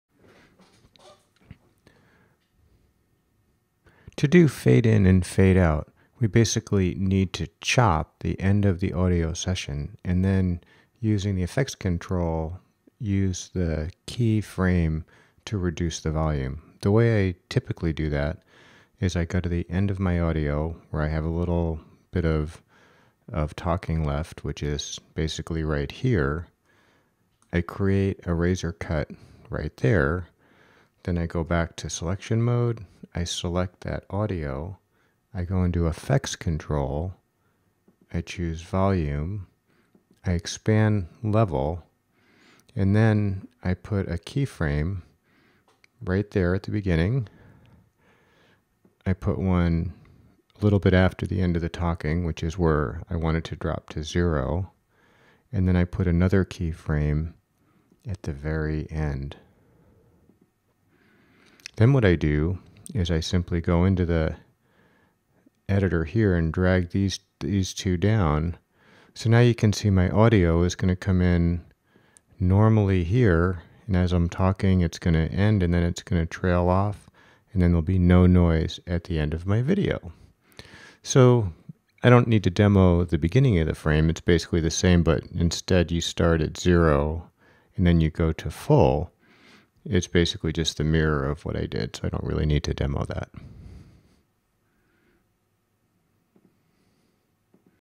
Adding Audio Fade in and out to a Video Clip in Adobe Premiere Pro
One high class move you can take to make your videos sound more professional is to fade in and fade out the audio at the start and end of your videos.

This short video (90 seconds) shows you how to easily do that.  If you like this clip, take a look at how to wrangle the timeline in Premiere Pro here: